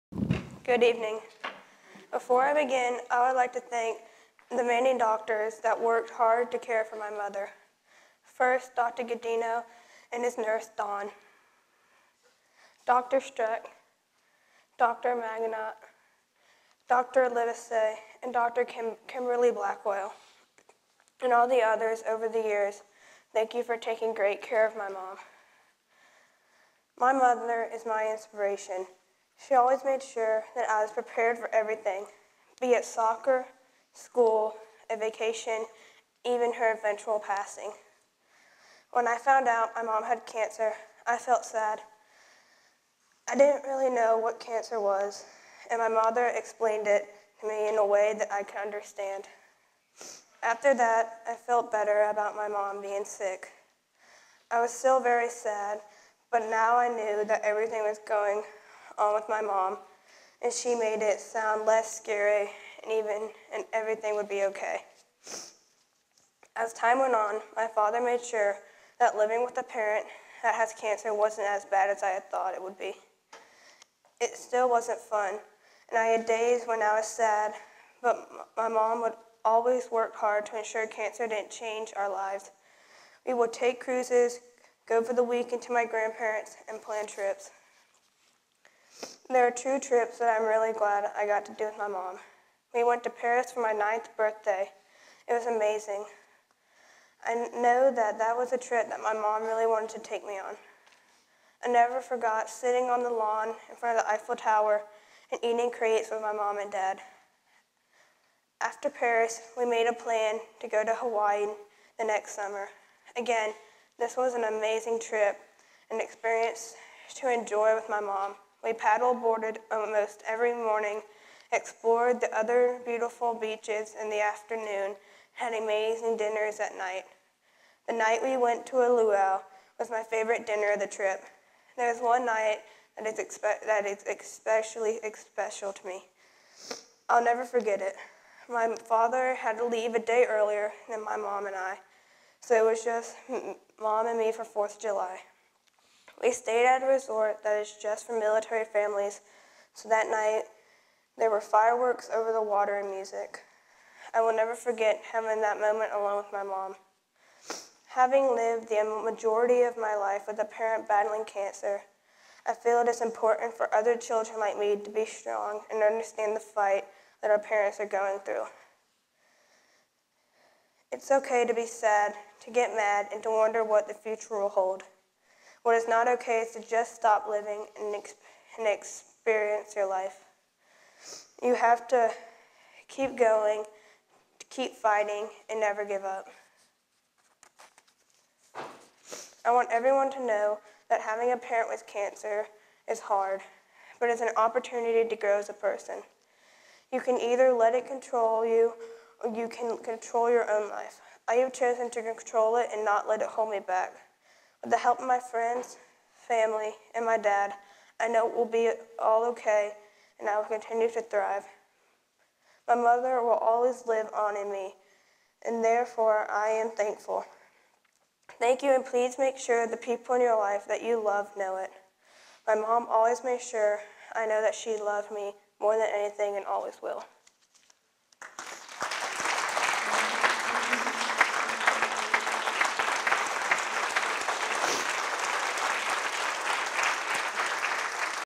Cancer took her mother but not her memories
Young lady talks about her mother she lost to Cancer